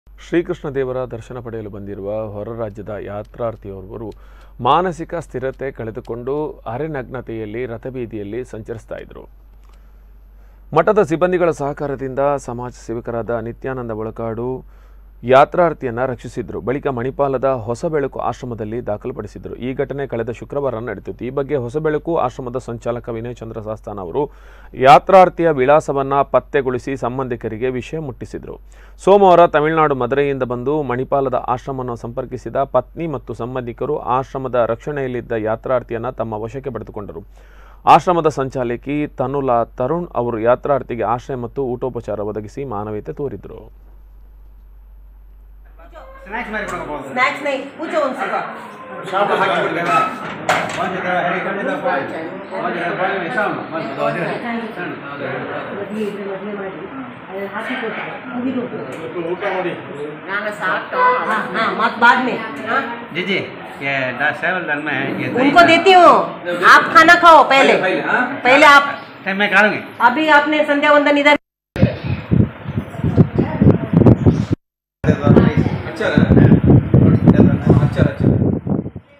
ಶ್ರೀಕೃಷ್ಣ ದೇವರ ದರ್ಶನ ಪಡೆಯಲು ಹೊರ ರಾಜ್ಯದಿಂದ ಬಂದ ಅನಾರೋಗ್ಯಪೀಡಿತ ವ್ಯಕ್ತಿಯ ರಕ್ಷಣೆ
ಶ್ರೀಕೃಷ್ಣ ದೇವರ ದರ್ಶನ ಪಡೆಯಲು ಬಂದಿರುವ, ಹೊರರಾಜ್ಯದ ಯಾತ್ರಾರ್ಥಿಯೊರ್ವರು ಮಾನಸಿಕ ಸ್ಥಿರತೆ ಕಳೆದುಕೊಂಡು, ಅರೆನಗ್ನತೆಯಲ್ಲಿ ರಥಬೀದಿಯಲ್ಲಿ ಸಂಚರಿಸುತ್ತಿದ್ದರು. ಮಠದ ಸಿಬ್ಬಂದಿಗಳ ಸಹಕಾರದಿಂದ, ಸಮಾಜಸೇವಕರಾದ ನಿತ್ಯಾನಂದ ಒಳಕಾಡು, ತಾರಾನಾಥ್ ಮೇಸ್ತ ಶಿರೂರು ಅವರು ಯಾತ್ರಾರ್ಥಿಯನ್ನು ರಕ್ಷಿಸಿದ್ದರು